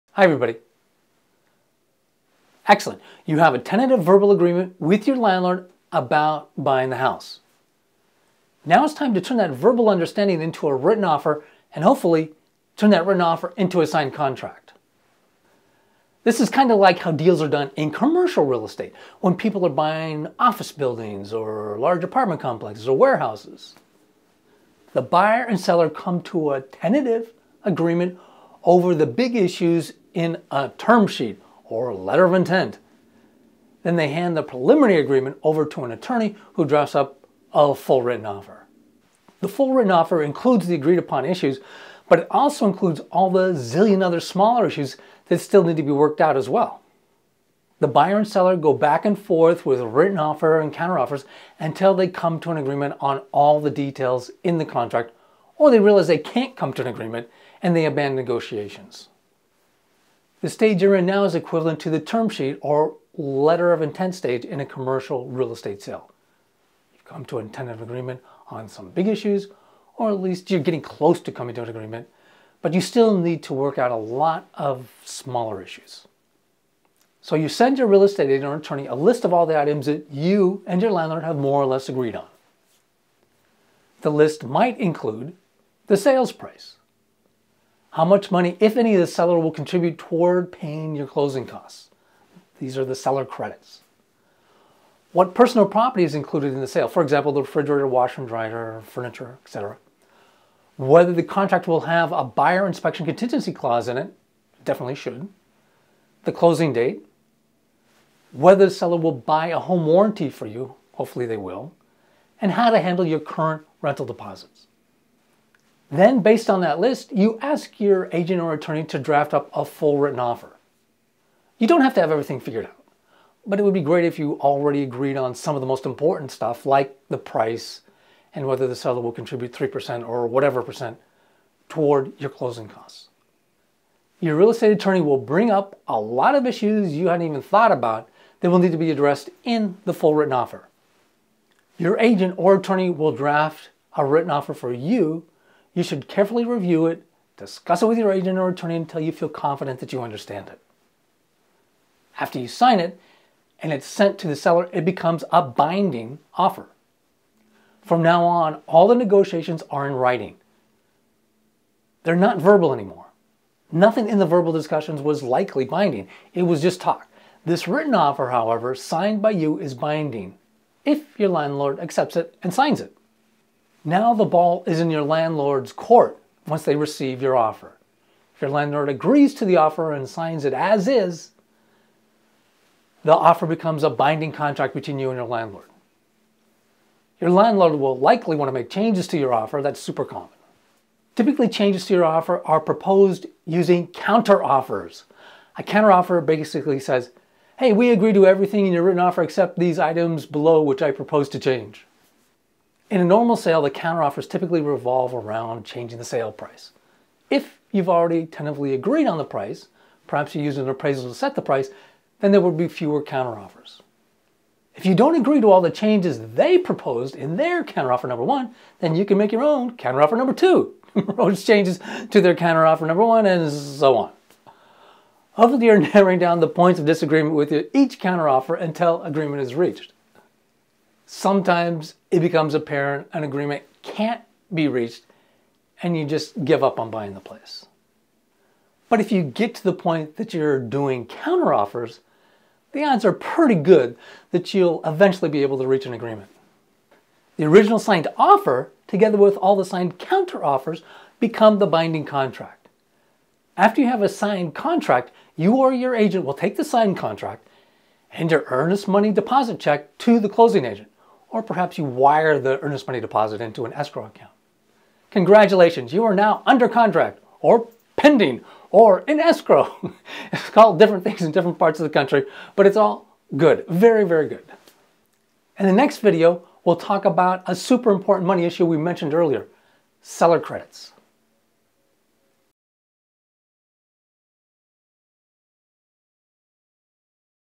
From Tentative Agreement to Written Offer to Signed Contract (Video 9.1)
"How to Buy the House You're Now Renting" Video Course
Chapter 9: Contract Terms
Video 1: From Tentative Agreement to Written Offer to Signed Contract

John Wake
Real Estate Decoded